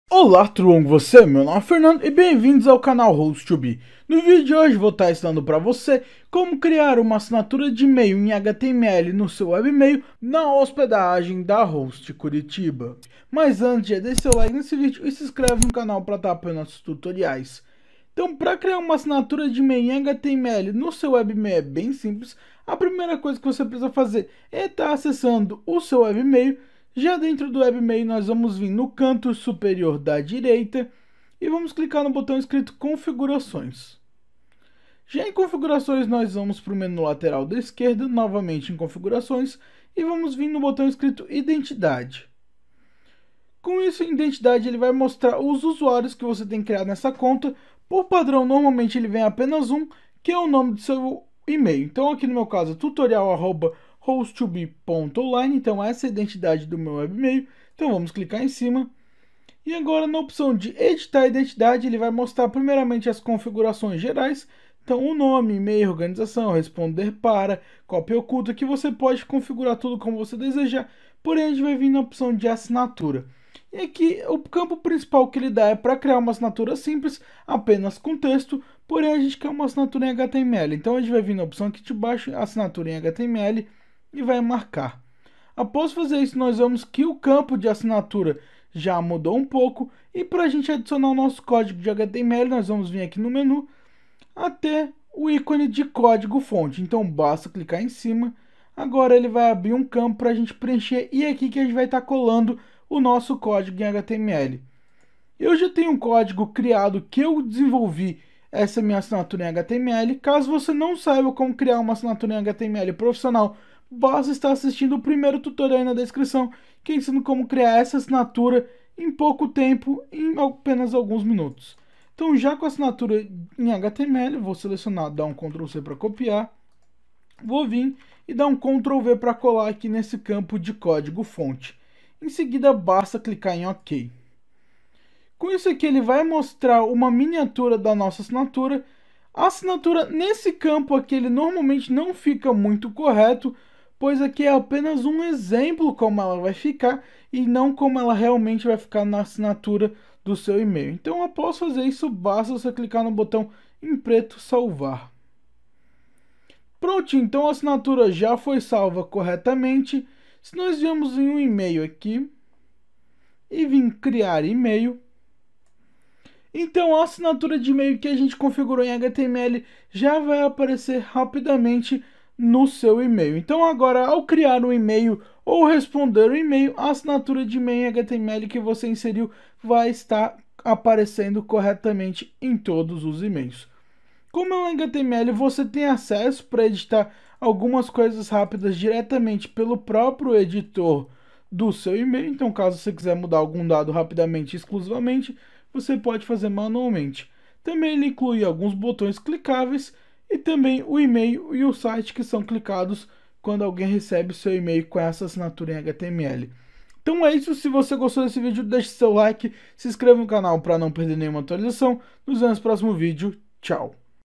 Como Criar uma Assinatura de Email HTML na Hostcuritiba – rápido e fácil – 2022
Aprenda Como Criar uma Assinatura de Email HTML na Hostcuritiba facilmente e em poucos minutos